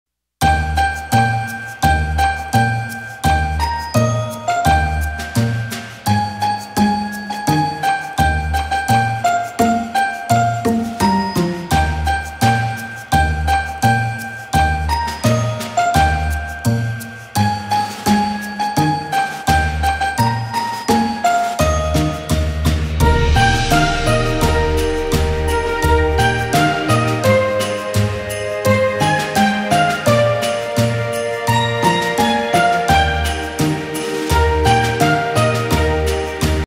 Make This Jingle Bell Shadowbox in 3 Easy Steps
If you're looking for a super easy and cute DIY Christmas gift for someone on your list, make this shadowbox in less than 20 minutes with these three easy steps!